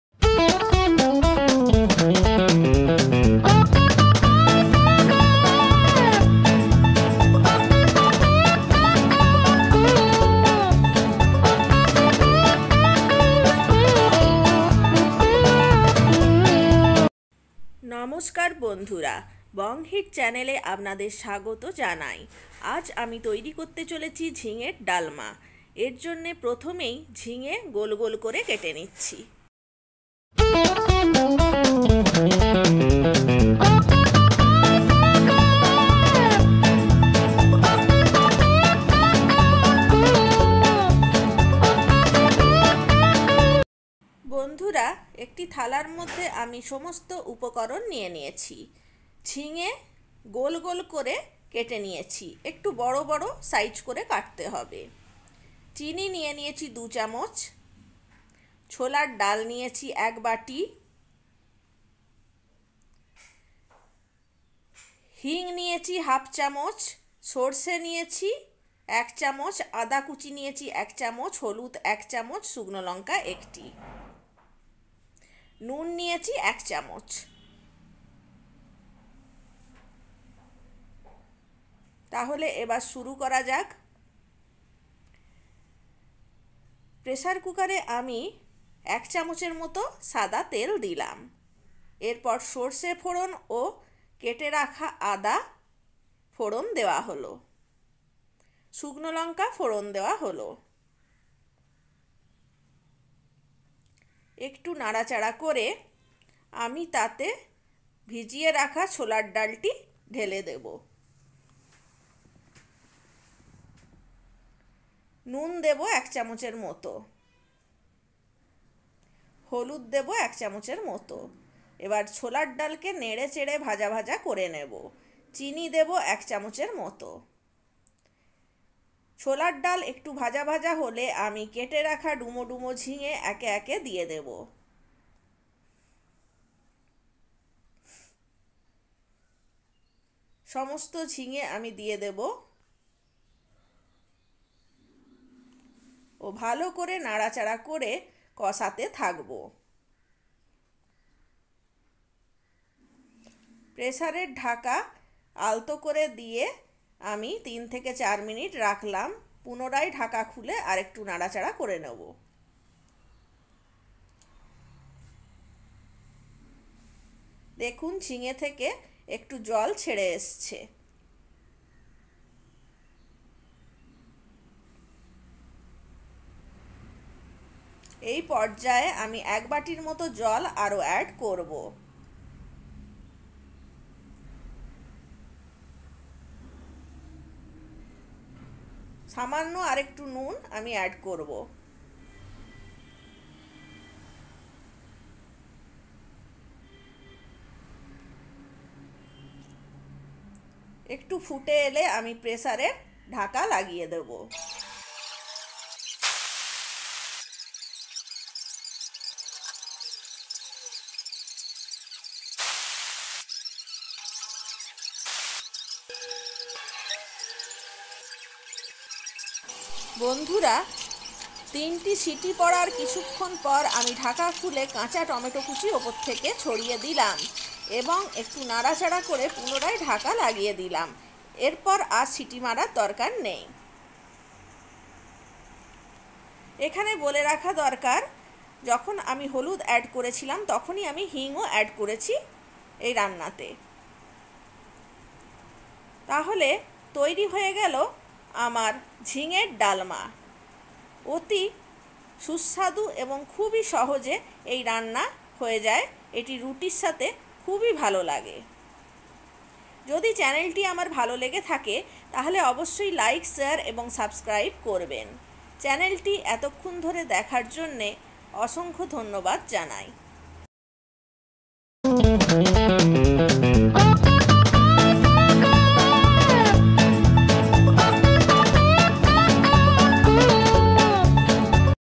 ঝিঙের ডালমা এই ভাবে করে গরম গরম রুটির সাথে একটি বার খেয়ে দেখুন
ঝিঙে চাকা চাকা করে কাটা ৬০০ গ্রাম, ছোলার ডাল ১৫০ গ্রাম, সরষে ১চামচ, আদা কুচি ১ চামচ, হী্ং ১/২ চামচ, হলুদ ১চামচ, গোটা শুকনো লঙ্কা ১টি, নুন ১চামচ, চিনি ২ চামচ, তৈল ১ চামচ

বিশেষ দ্রষ্টব্য - ভিডিও তে ভুল করে ১ চামচ চিনি বলা হয়েছে ওটা ২ চামচ হবে